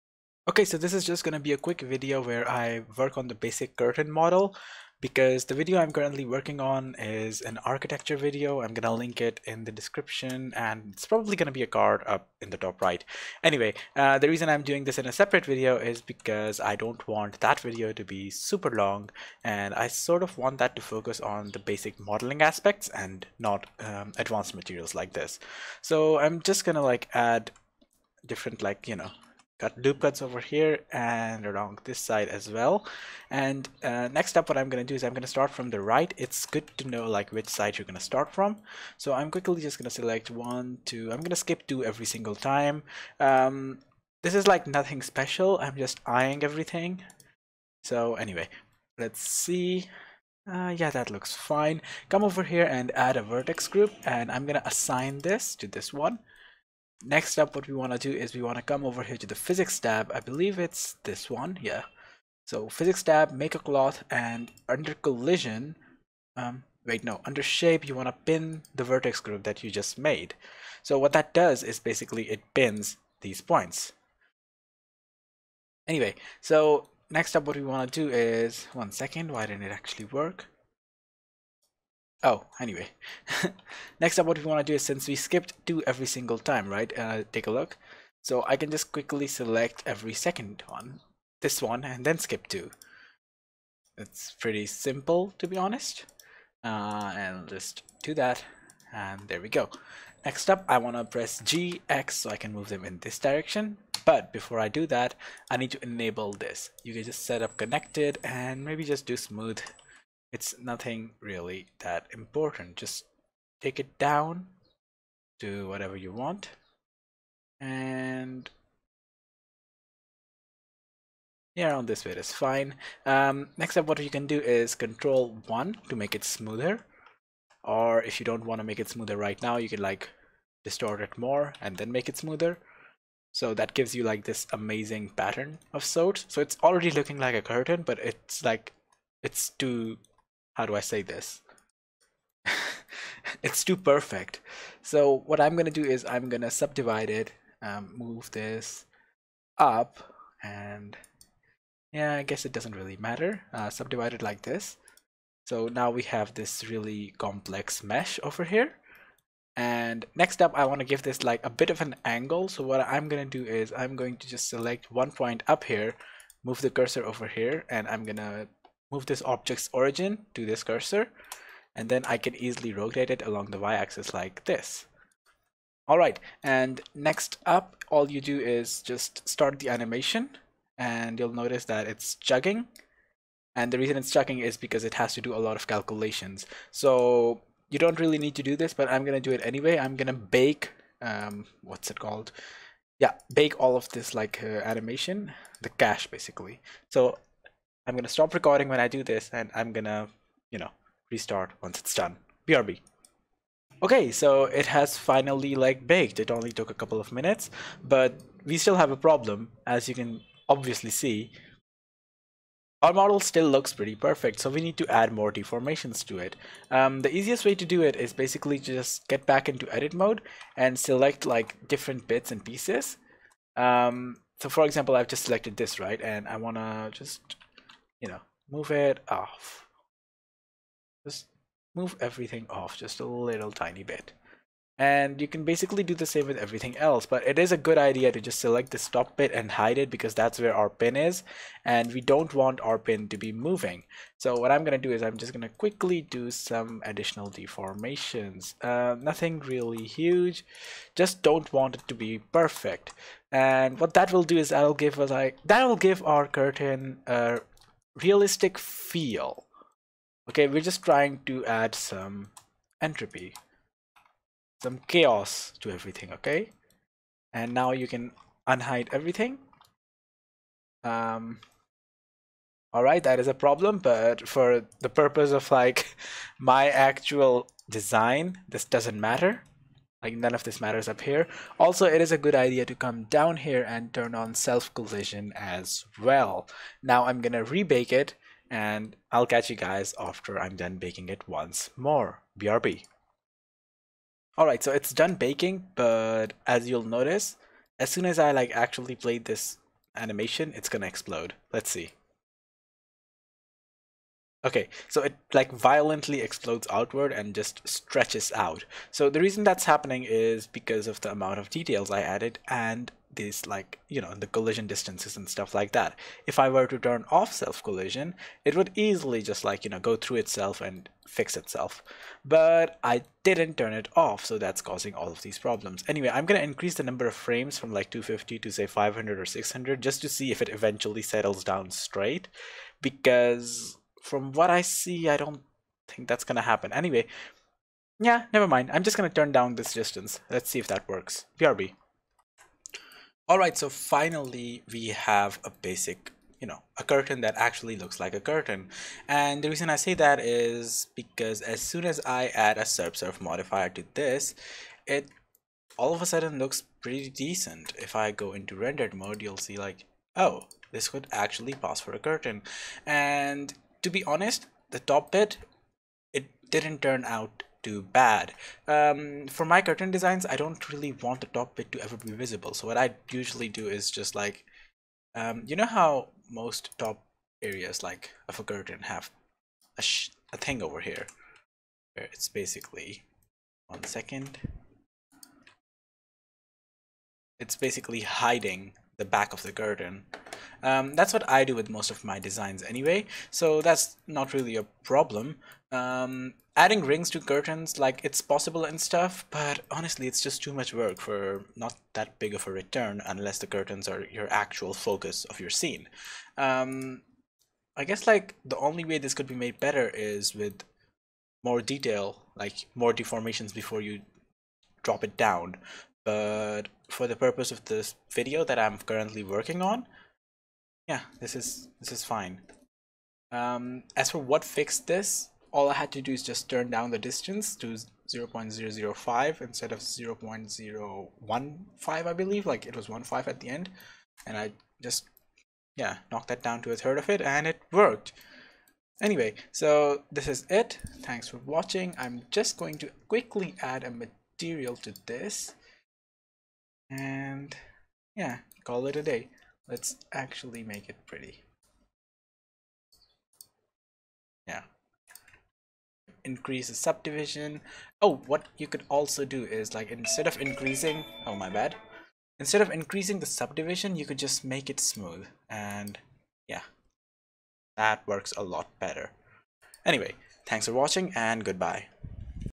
Simple Curtains - Quick Model Tutorial - Blender 2.8
This is a quick curtain modeling tutorial where I use Blender's cloth simulator to generate realistic distortions. I'll be using this model for my Bedroom scene, if you're interested in that then check the playlist for that:

All of this is done as quickly as possible! Check out the playlist if you're interested in more 3D models, I'll be updating it as I publish more videos.

Let me know if I did anything wrong! There's always room for improvement!